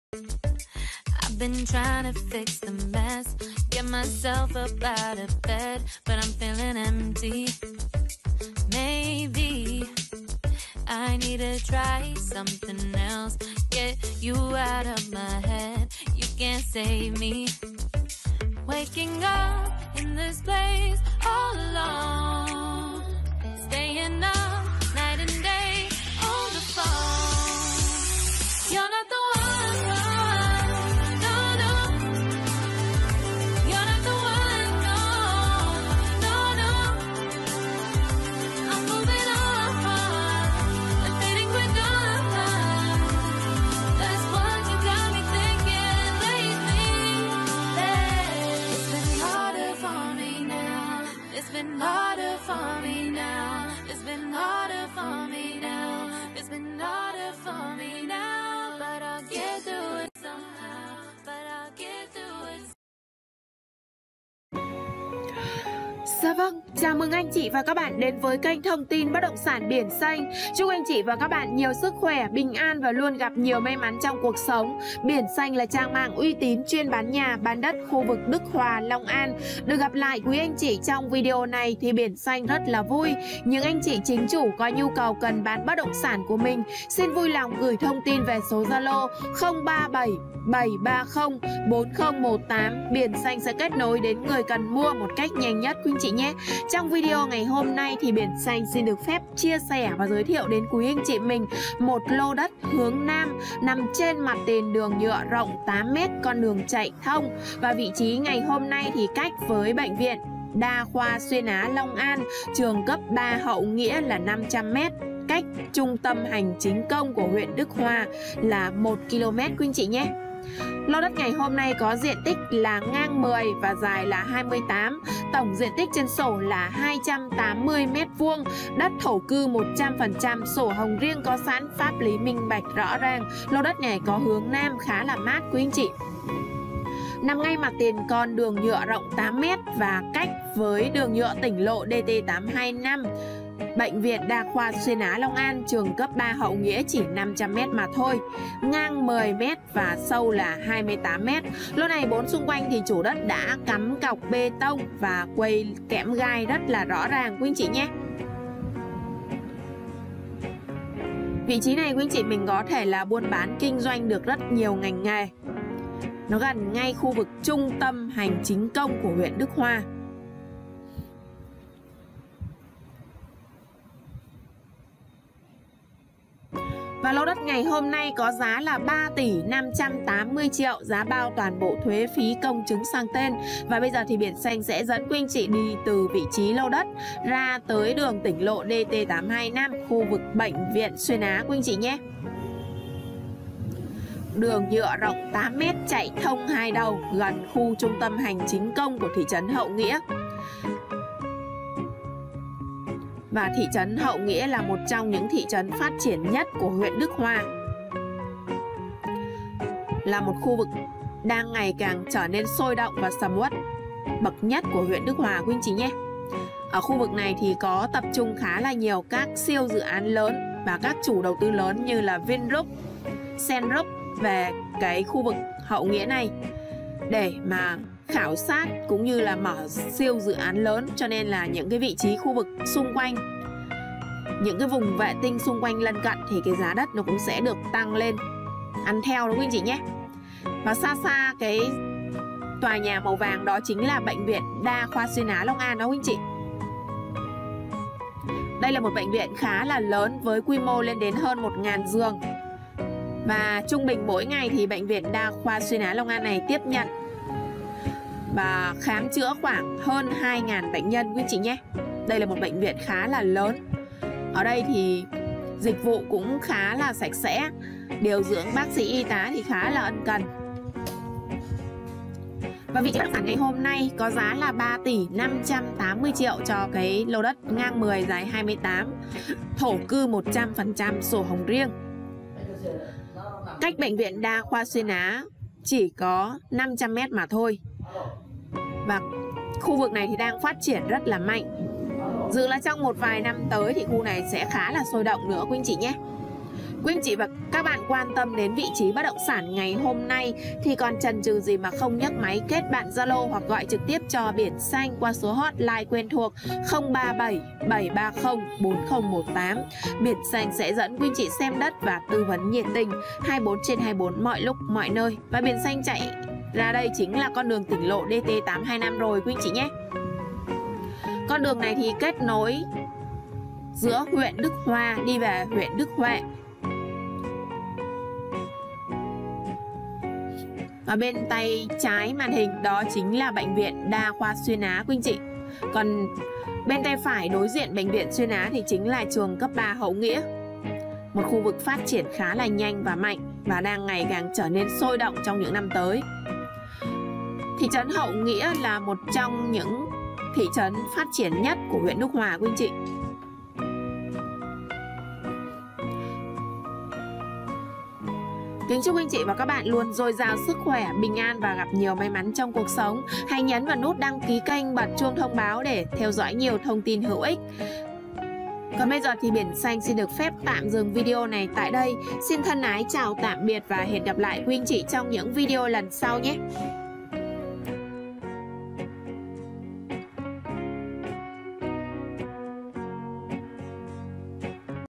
Đất Mặt Tiền Đường Nhựa ,Ngay Bệnh Viện Xuyên Á 10x28m Thổ Cư Trung Tâm Hậu Nghĩa
💎👉Kênh này Bản quyền được bảo vệ và quản lý bởi  Network 👈💎

# Biển Xanh kính Chúc Quý Anh Chị và Các nhà Đầu Tư mỗi ngày luôn vui vẻ, hạnh phúc và bình an nhé !